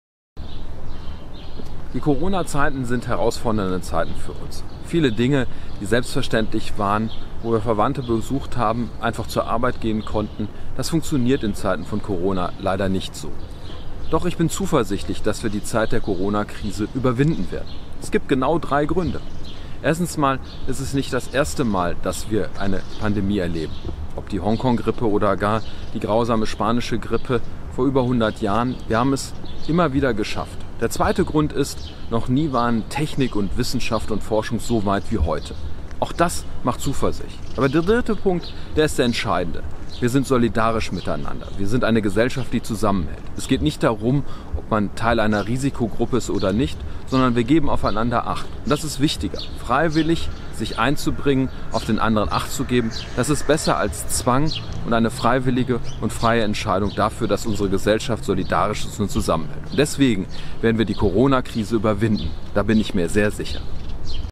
Sebastian Hartmann zur Corona-Krise
Sebastian Hartmann ist Landesvorsitzender der SPD in Nordrhein-Westfalen und seit 2013 Mitglied des Deutschen Bundestages. In diesem kurzen Video äußert er sich optimistisch zur Corona-Krise.